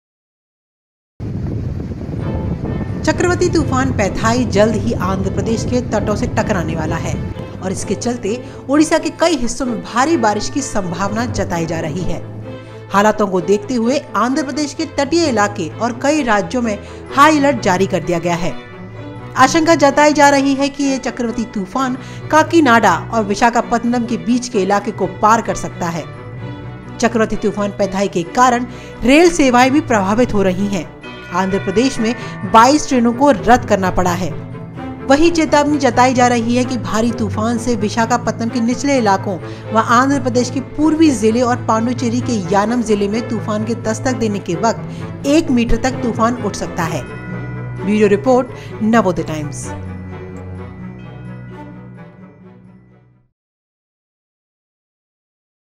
Andhra Pradesh में 'पेथाई' चक्रवाती तूफान का खतरा, कई राज्यों में High-Alert जारी I Cyclone Phethai
चक्रवाती तूफान पेथाई जल्द ही आंध्र प्रदेश के तटों से टकराने वाला है और इसके चलते ओडिशा के कई हिस्सों में भारी बारिश की संभावना जताई है. हालातों के दिखते हुए, आंध्र प्रदेश के तटीय इलाके और कई राज्यों में हाई-अलर्ट जारी कर दिया गया है...